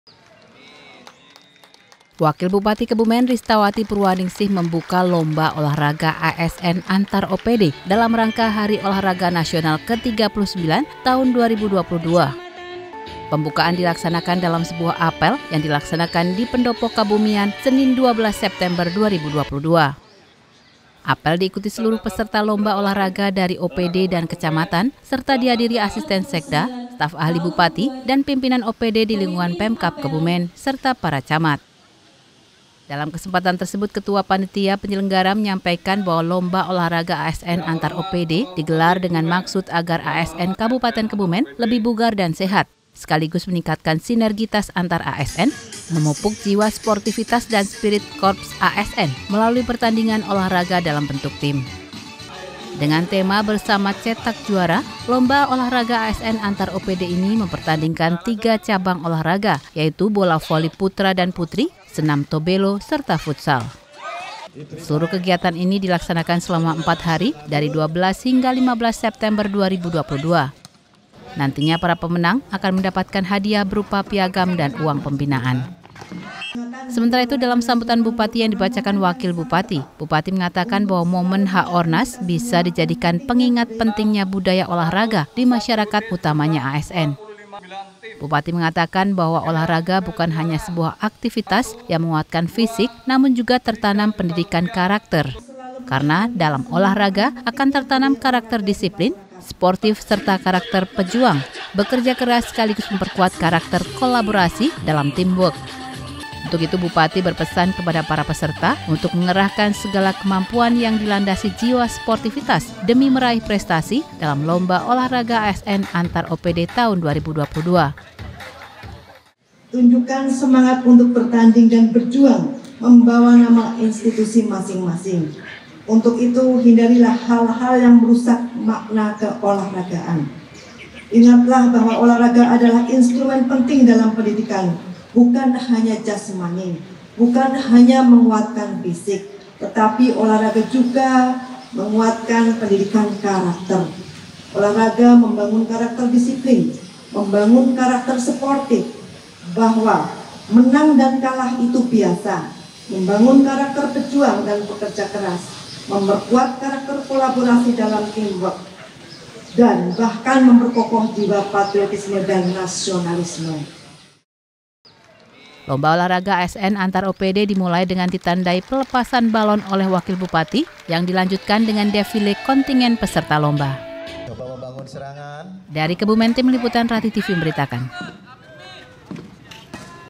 Peringati Haornas, Pemkab Kebumen Gelar Lomba Olahraga Antar-OPD
Berita Kebumen Edisi Senin, 12 September 2022

Pemkab Kebumen menggelar lomba olahraga ASN antar-OPD untuk memperingati Hari Olahraga Nasional Tahun 2022. Lomba yang dibuka Wakil Bupati Ristawati Purwaningsih, agar ASN lebih bugar dan sehat, serta meningkatkan disipilin, sinergitas dan sportivitas.